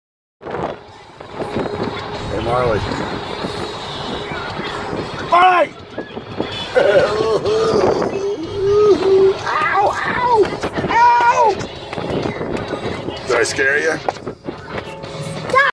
March 14, 2017
Trying to get Marley's attention for a picture.  Lol